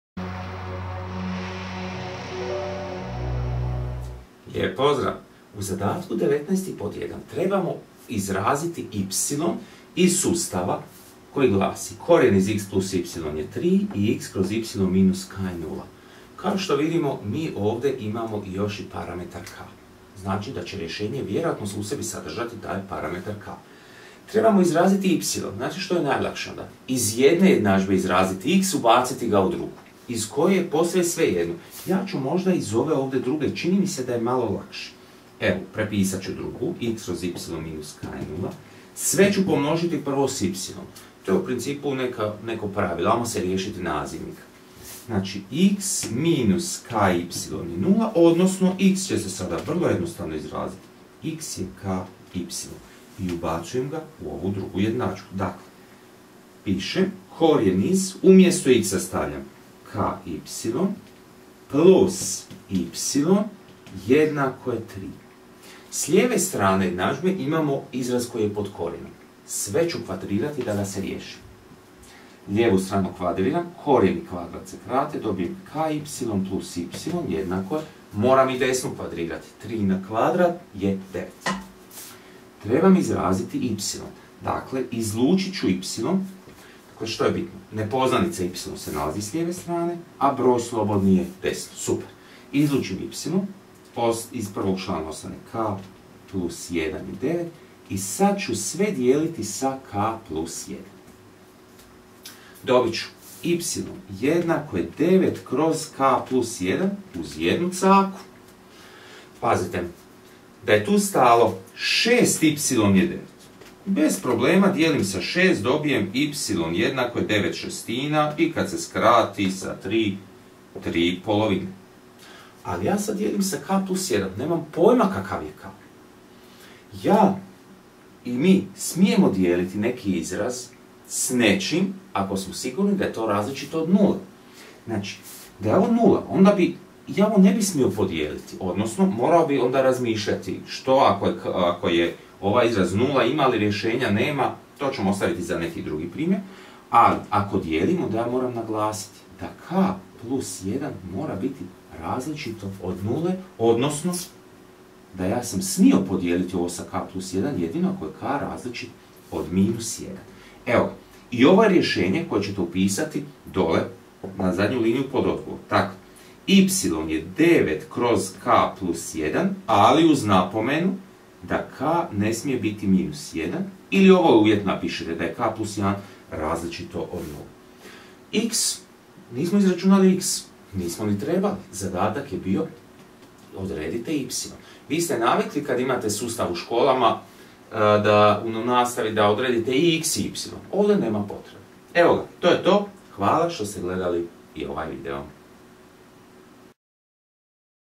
Državna matura matematika 19.1 zadatak VIŠA RAZINA ljeto 2012. SUSTAV JEDNADŽBI
y u rješenju sustava sqrt(x+y)=3    x/y-k=0